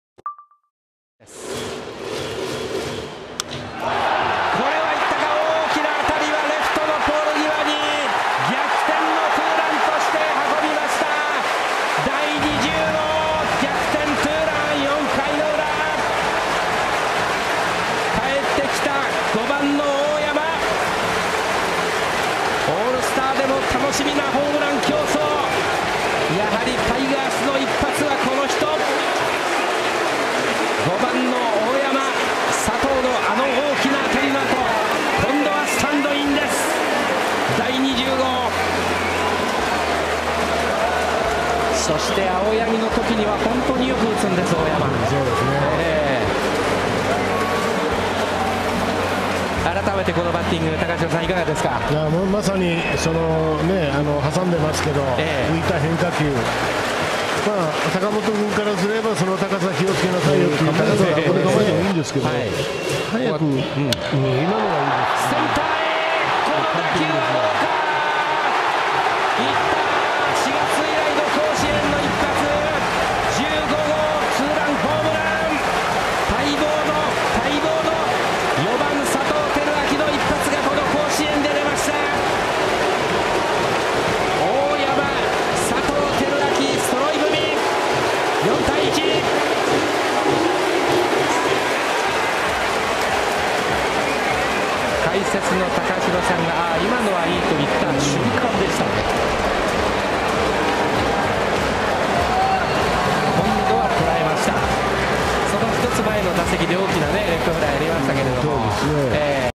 【祝砲】大山悠輔、佐藤輝明アベックアーチ 7/22vs Dena戦
待ってました大山悠輔、佐藤輝明のアベックアーチ！この2人が打つとやはり盛り上がります。残り試合も期待しております。